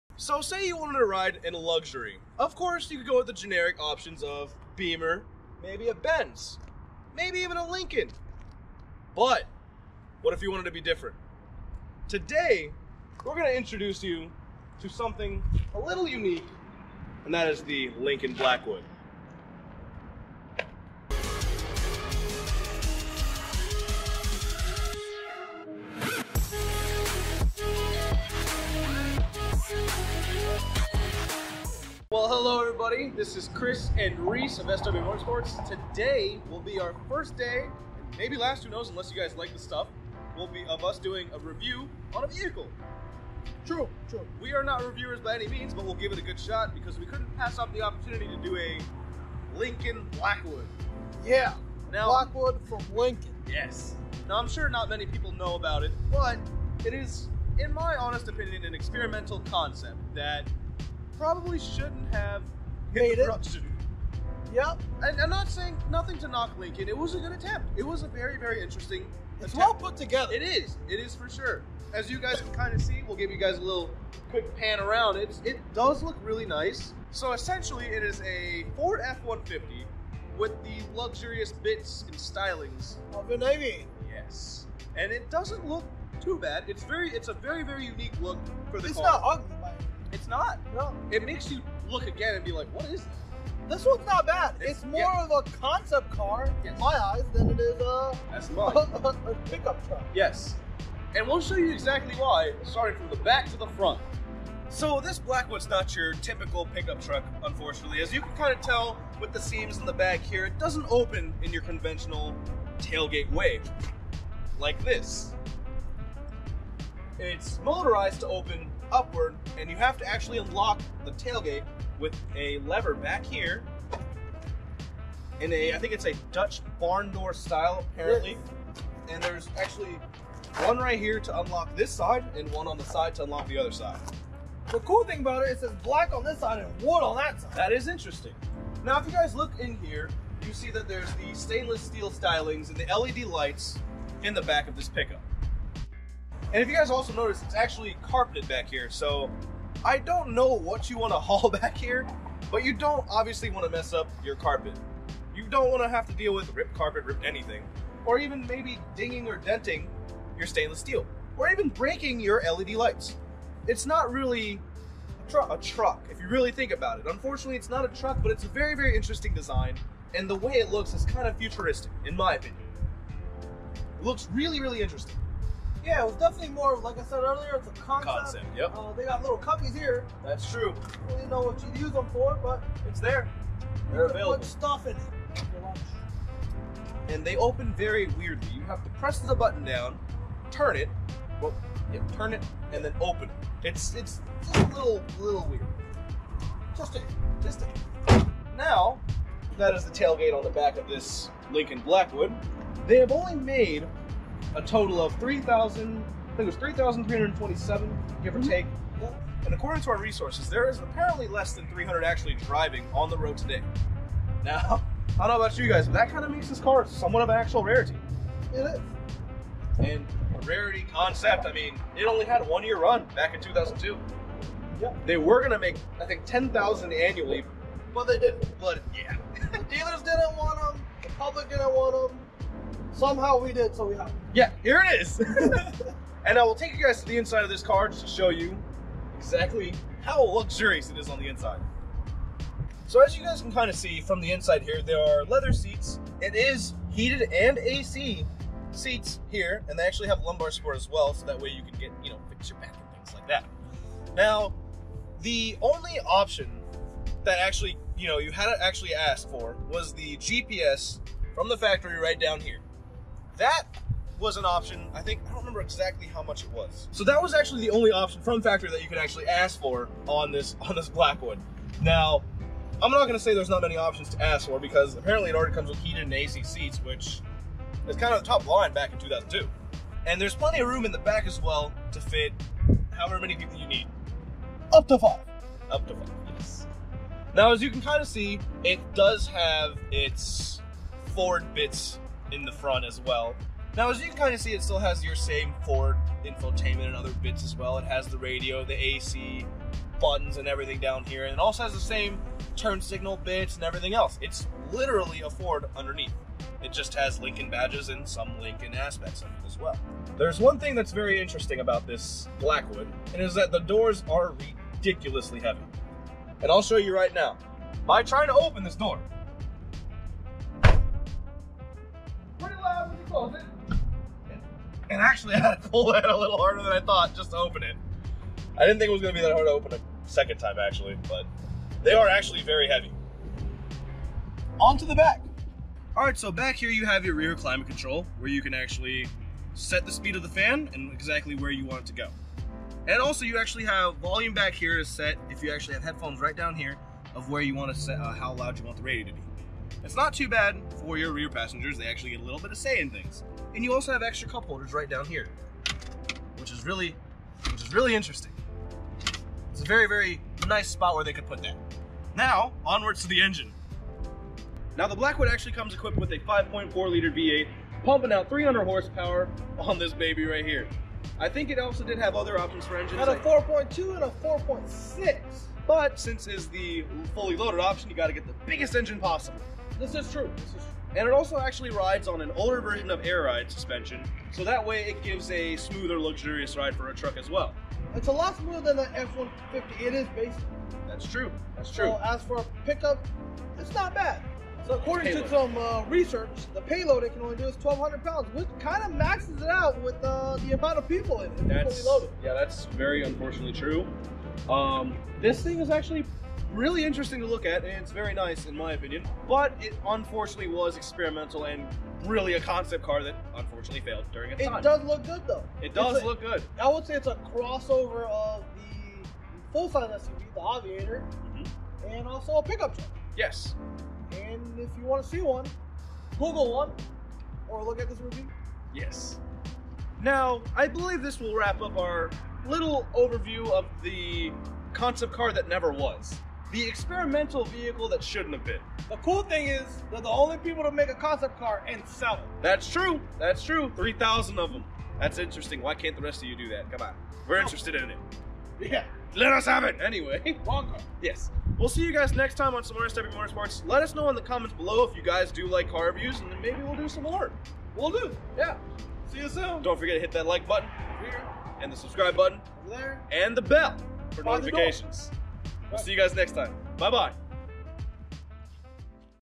2002 Lincoln Blackwood An Attempt for a Luxury Truck?
Hey everybody, we hope you enjoy our first car review starting with this Lincoln Blackwood! Its a very interesting truck which we believe was the first step into a Luxury truck. Does its one year run really make it a rarity? Be sure to watch our video and find out!